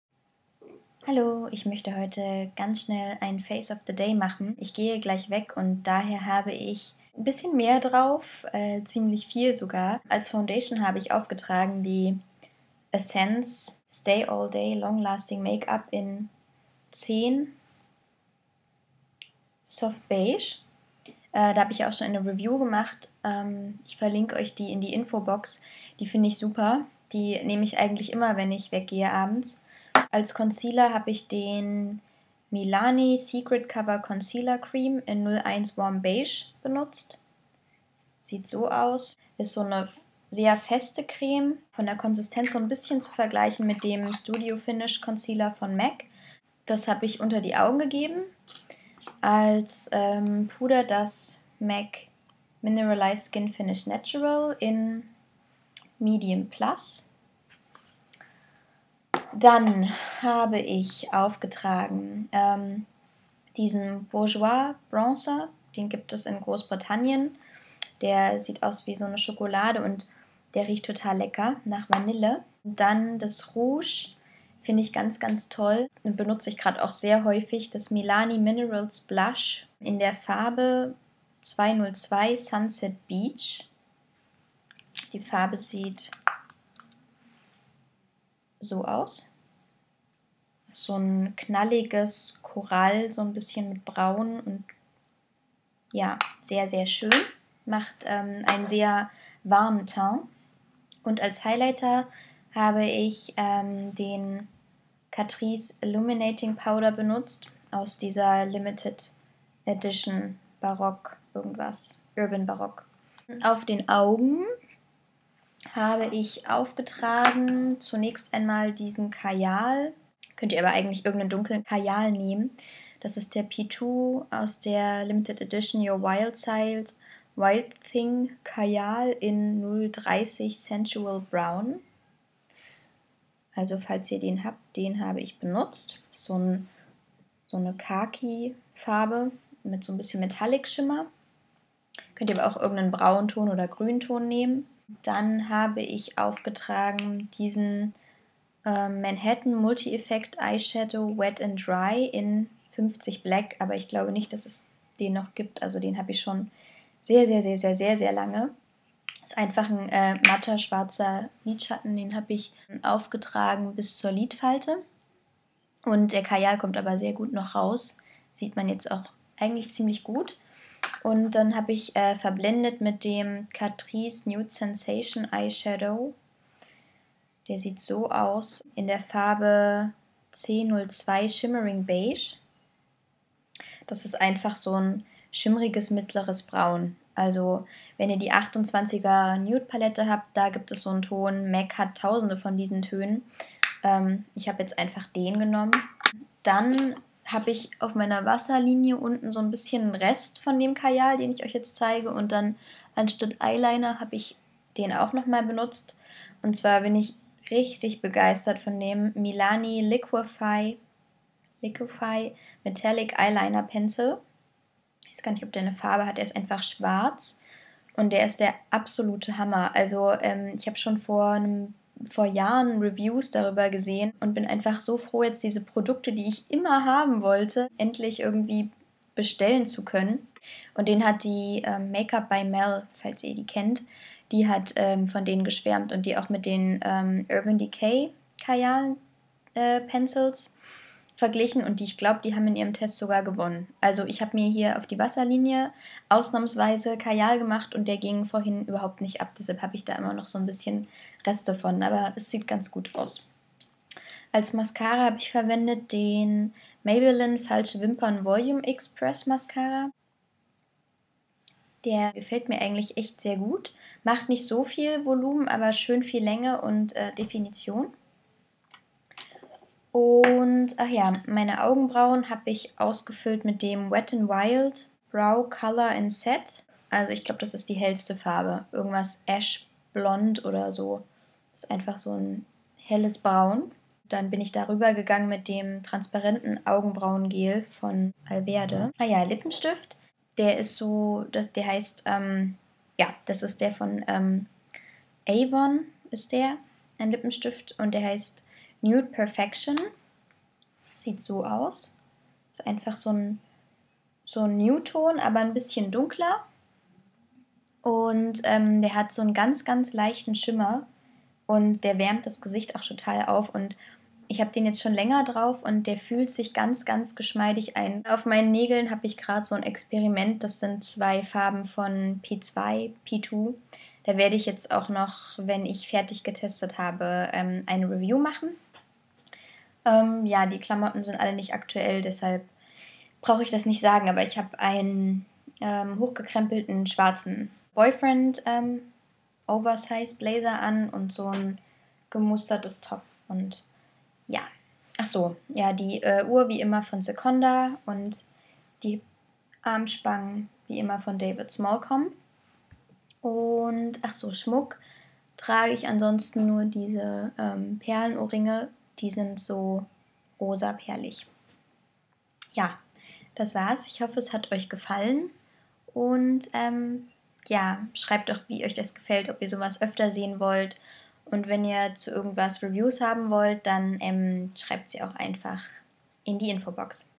FOTD 5. April 2011
Verwendete Produkte:
+ Essence Stay All Day make Up in 10 Soft Beige
+ Milani Secret Cover Concealer Cream in 01 Warm Beige
+ MAC MSF Natural in Medium Plus
+ Bourjois Bronzer
+ Milani Minerals Blush in 202 Sunset Beach
+ Catrice Illuminating Powder aus der Urban Baroque LE
+ P2 Kajal in 030 Sensual Brown aus der Your Wild Side LE
+ Manhatten Multi Effect Wet&Dry Lidschatten in 50 Black
+ Catrice Nude Sensation Lidschatten in Co2 Shimmering Beige
+ Milani Liquif'eye Mettalic Eye Liner Pencil in 01 Black
+ Maybelline Falsche Wimpern Volum'Express Mascara
+ Wet'n'Wild Augenbrauenstift in Ash Blond(?)
+ Alverde Augenbrauengel
+ Avon Lippenstift in Nude Perfection

+ P2 Nagellacke in 100 Open Your Heart und 690 Forever

+ Boyfriendblazer von Primark
+ gemustertes Longtop von H&M
+ Uhr von Sekonda
+ Armspangen von David Smallcomb
+ Ohrstecker von H&M